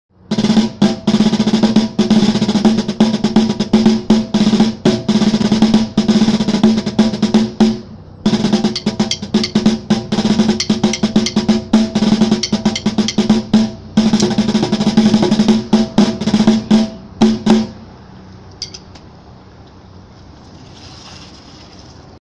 Remo Rhythm Pal!
Vance Nurkala demos the new Remo Rhythm Pal head (and snare fixture) on a cheap pail!  Incredibly inexpensive (and lightweight) marching drum.  Great for street groups, kids marching bands, etc.  drumexchange.com